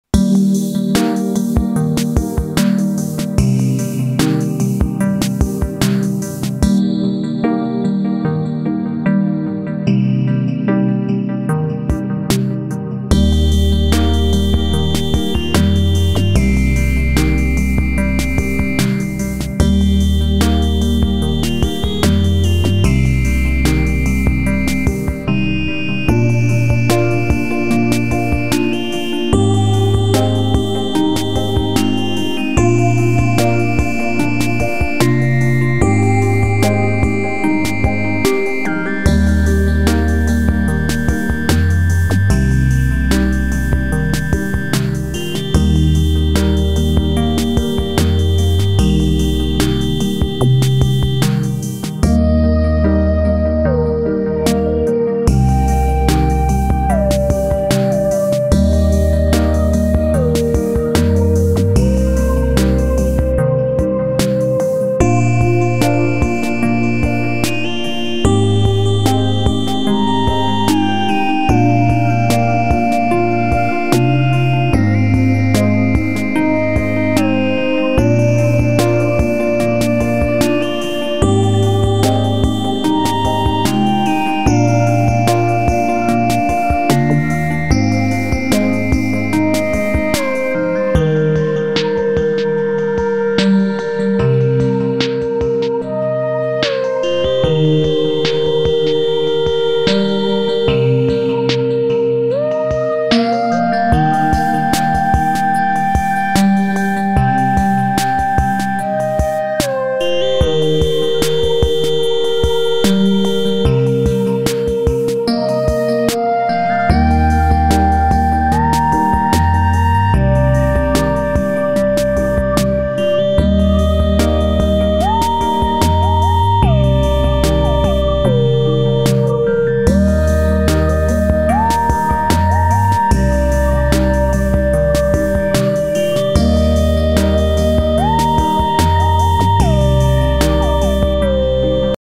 Thunder Bay Drive weather is getting nicer out, soon we can do a bike ride
Short drive to get dog food.
Towards County Fair Plaza Walmart Dawson Road Area,